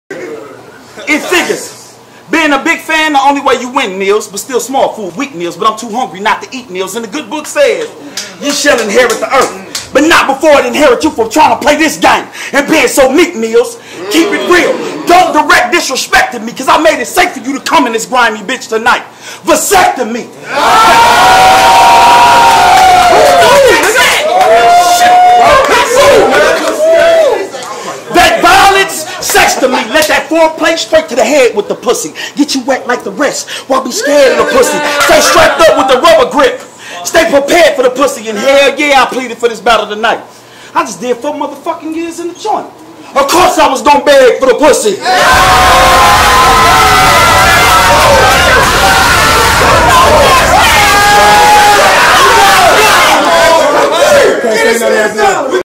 noChill noFilterTV | Great Britton Quick Clip
This is a noChill noFilterTV  Quick Clip of Great Britton barkin them atomic bars at Team Extendo's Mista Milll$ courtesy of noChill noFilterTV